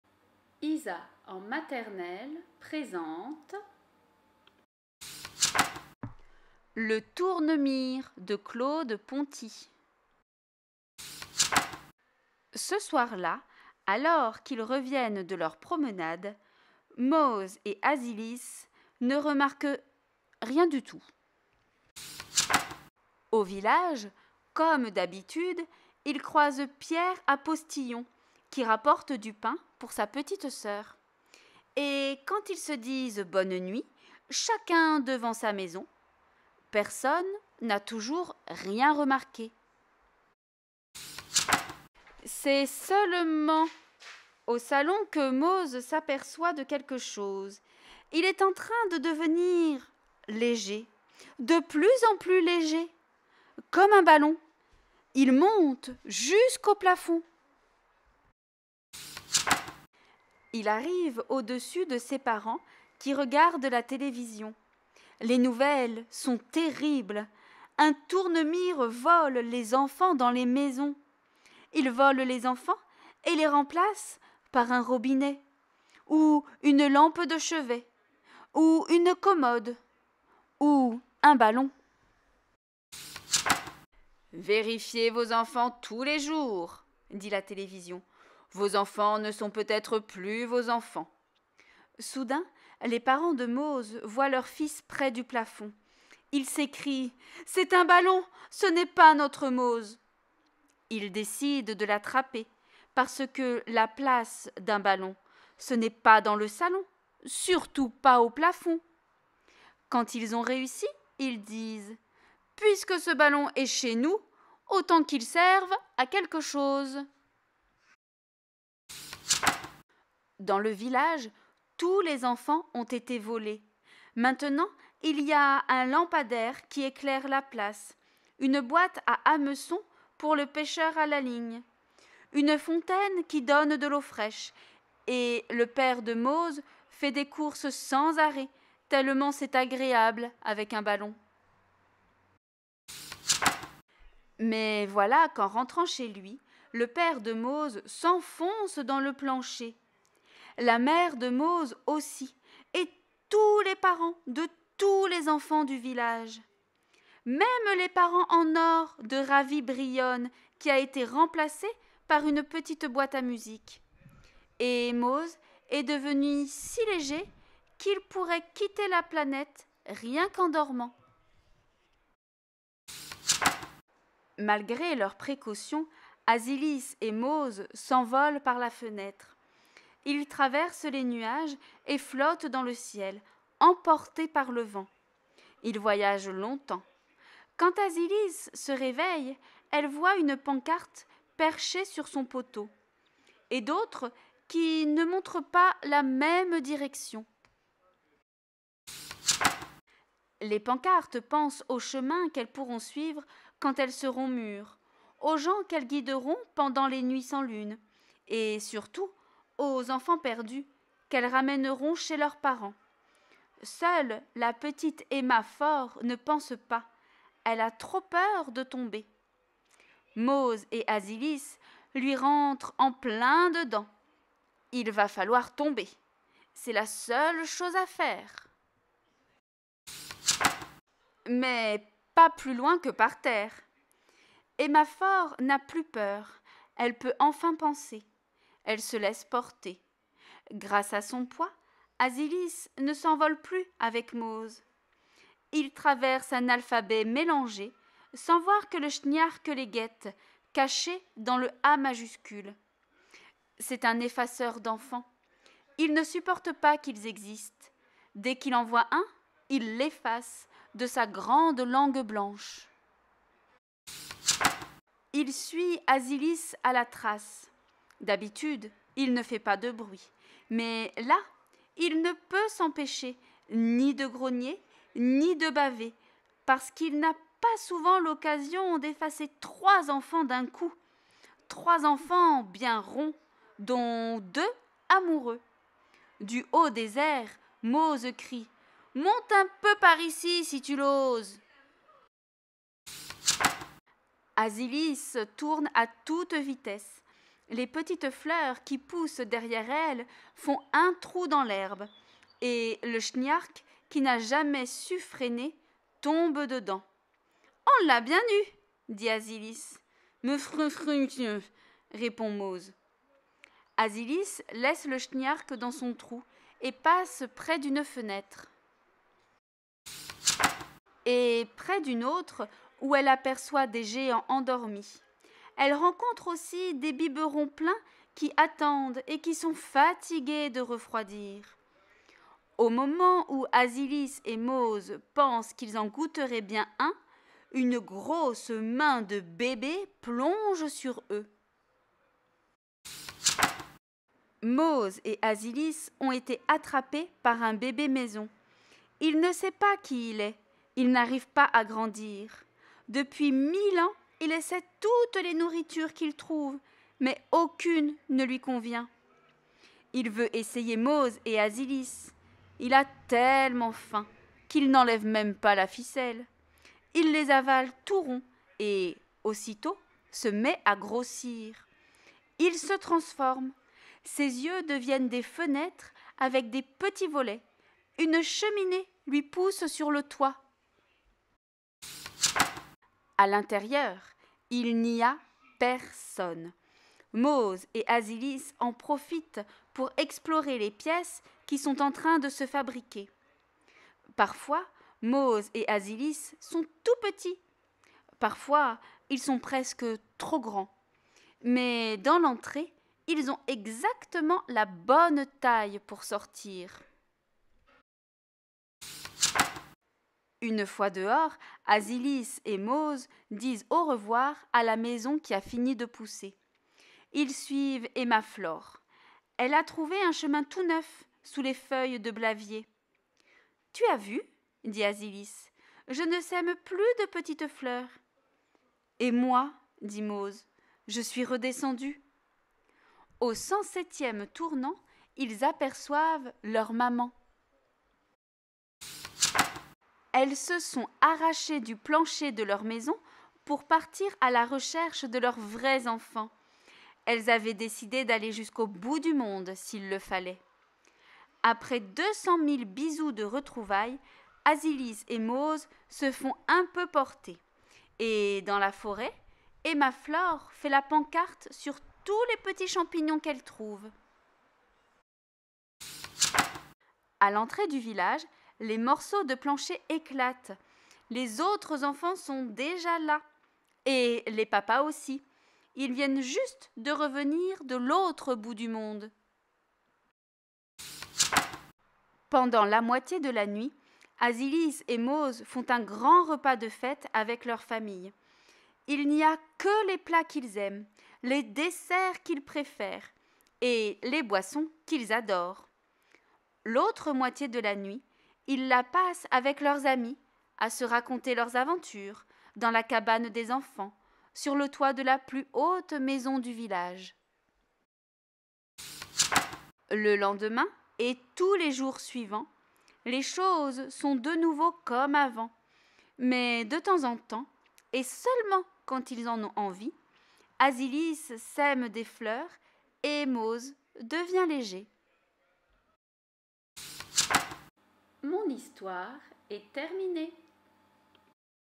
Le tournemire, Claude Ponti
Une histoire de village où les enfants sont transformés.